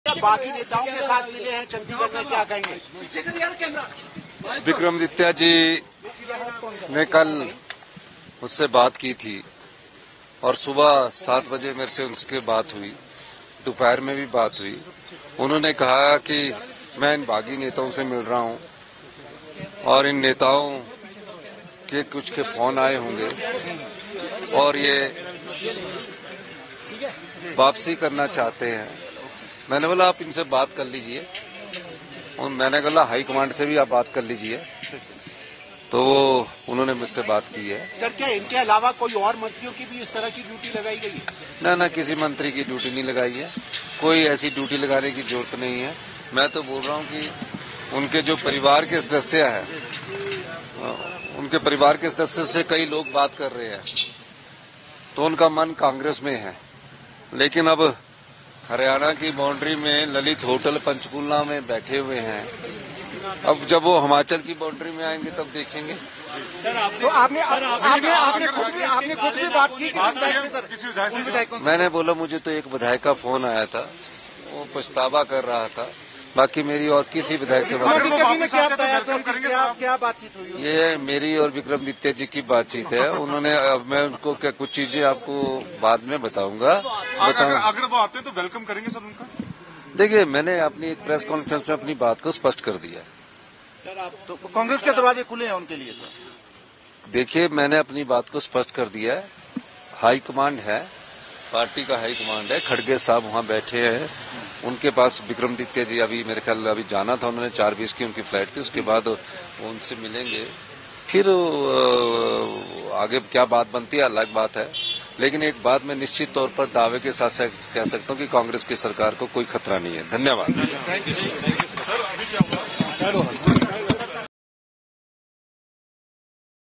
Himachal Pradesh News: CM Sukhvinder Singh Sukhu ने बागी विधायकों को लेकर कह दिया बड़ा बात | Aaj Tak
हिमाचल प्रदेश की राजनीतिक स्थिति पर मुख्यमंत्री सुखविंदर सिंह सुक्खू ने कहा, "विक्रमादित्य सिंह कल कैबिनेट में थे, उनसे हमारी बातचीत हुई है और कैबिनेट बैठक के बाद वह शाम को चंडीगढ़ गए जहां ललित होटल में वह हमारी बागी विधायकों से मिले। बागी विधायकों में से कुछ कांग्रेस में वापस आना चाहते हैं...मेरी उनसे सुबह और दोपहर में बात हुई है। मैंने उनसे कहा कि आप हाईकमान से बात कर ले। एक बागी विधायक ने मुझसे कहा था कि वह वापस आना चाहते हैं लेकिन वहां पर CRPF तैनात कर दिए गए हैं। तो हम ऐसा काम नहीं करने वाले जब उनका मन होगा वो वापस आए और हम उनका स्वागत करेंगे क्योंकि जोर-जबरदस्ती हिमाचल की संस्कृति में नहीं है..."